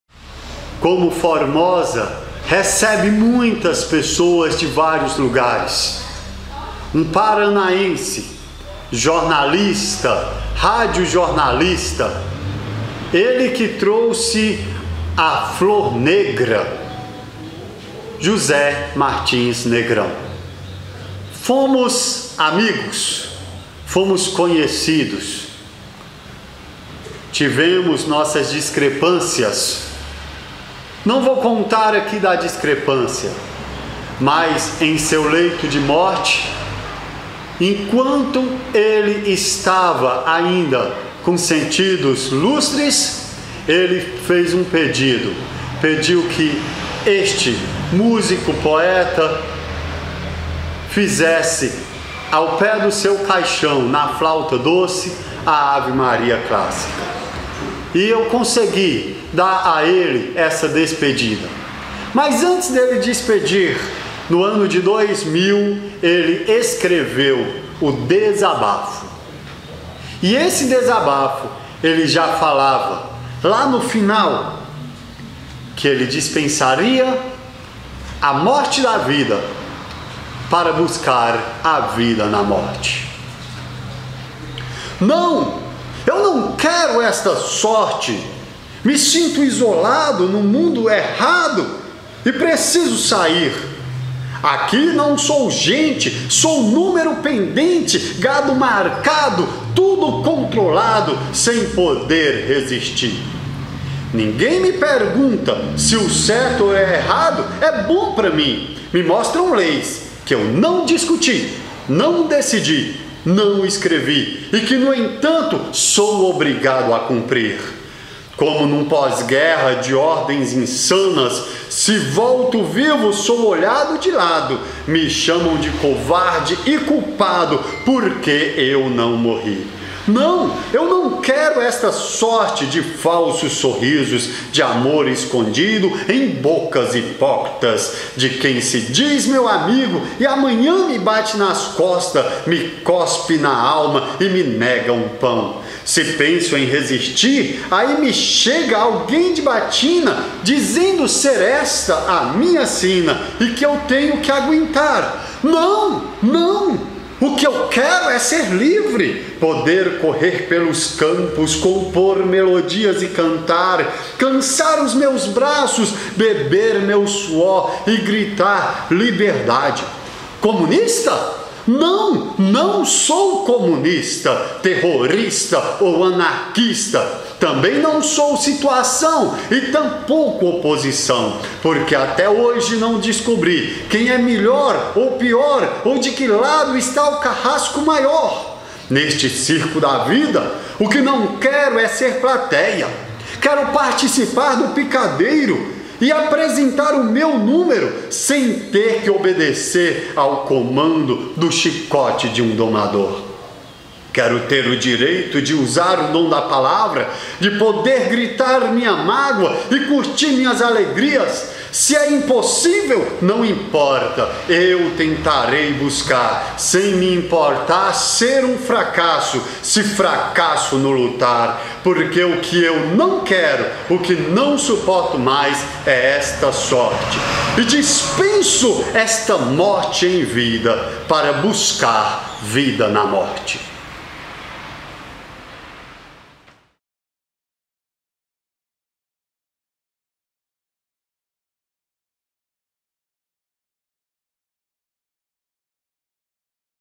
crônica poética - Desabafo - José Negrão - Formosa GO
Em Formosa GO temos um celeiro de novidades e, o jornalista formoseado José Martins Negrão deixou para a nossa memória essa crônica jornalística que aborda vários assuntos políticos, principalmente a obrigação de seguir leis que não foi ele quem criou.
Outra questão interessante é que poucos anos após ele dizer na crônica que estava abandonando a morte na vida para buscar vida na morte, esse escritor e jornalista realmente faleceu.